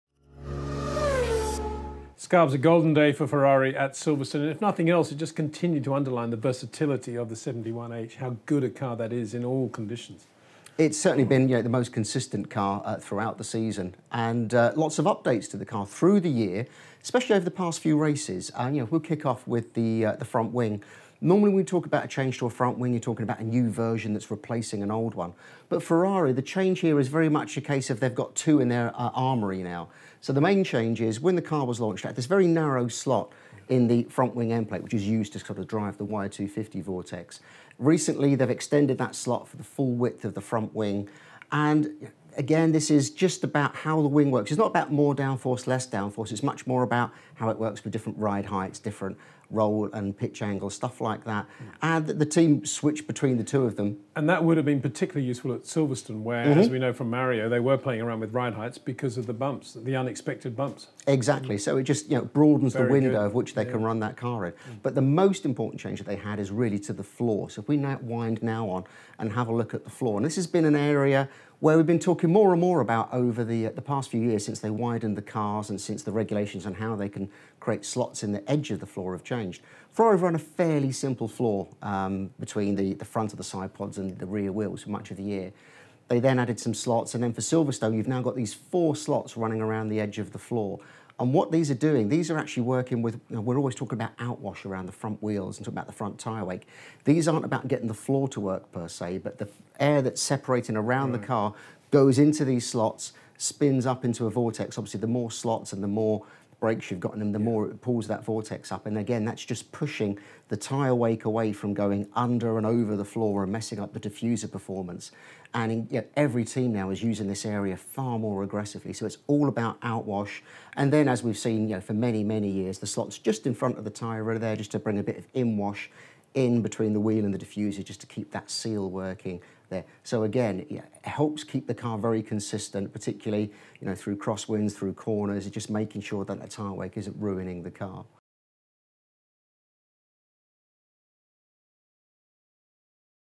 How new floor slots helped Ferrari win the British GP
Our exclusive 3D animation shows how the British GP-winning Ferrari SF71H utilised new floor slots to both minimise aero disruption caused by tyre wake and improve diffuser performance. Craig Scarbrorough explains...

After Silverstone, who do you think now looks stronger for the title?